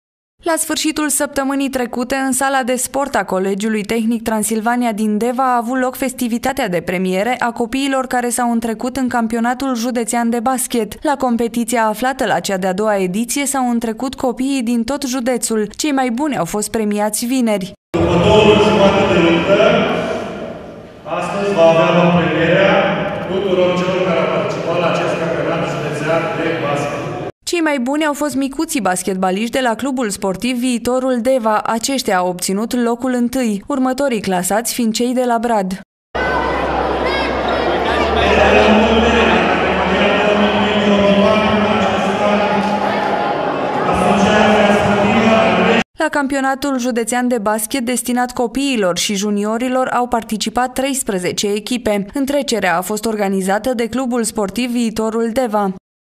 CAMPIONATUL JUDEȚEAN DE BASCHET A AJUNS LA FINAL
Festivitatea de premiere a avut loc vineri